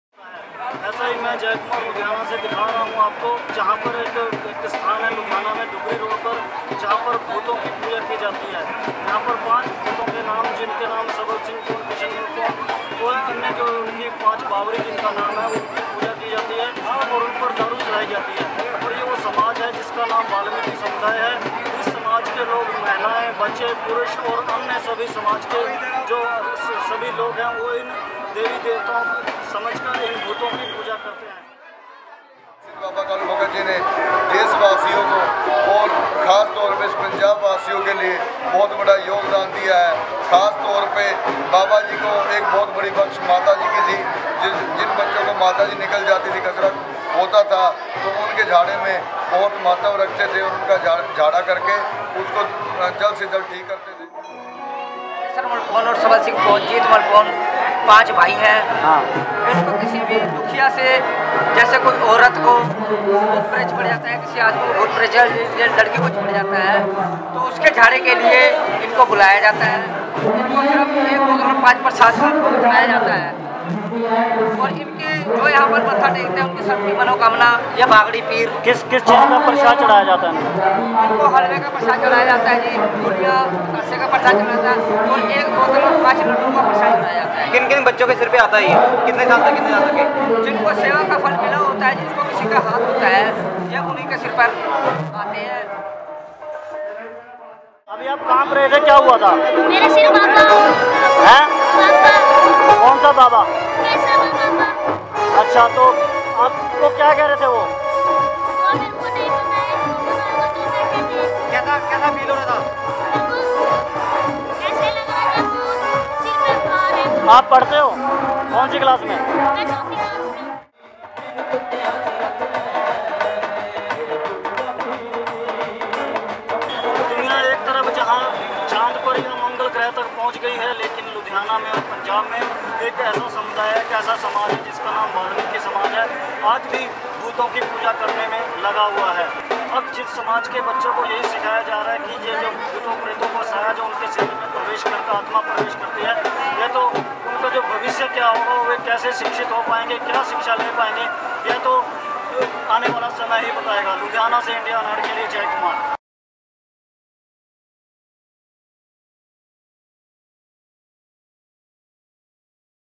Exorcising The Ghosts of Blind Faith
The Valmiki Community of Ludhiana is being tricked into superstition.
About The Video: The Valmiki Dalit Community of Ludhiana, Punjab is fervent in its belief that the spirits of the 5 saints, collectively known as 'Paanch Bavari' are guardians of their body and soul. Community Correspondent Jai Kumar, a Dalit himself, maintains that while culture is important for asserting the identity of the community, he worries that the 'belief' is being misused and abused and people are being led into the dark alleys of blind faith and superstition.

Fever and bodily ailments are being termed by quacks and the priests as ghostly possessions. The sufferers are subjected to physical and mental abuse in the name of 'exorcism'. Amidst burning pyres, chants, animal sacrifices, offerings of sweets and liquor, the patients are literally whipped into frenzy until they are held in a trance where
they begin to whip themselves with steel chains like mad dervishes before the idols of the saints.

Community Correspondent Jai Kumar says that what is shocking to him is the number of youths who are showing interest in these rituals. "I request the parents that they not force their own children into these beliefs. Dalits need enlightenment and critical thinking for their emancipation, not prostrating and flailing themselves in front of stone idols. Dalits themselves are the custodians and guardians of their community and their bodies and identities. It is not the responsibility of a bunch of long dead saints."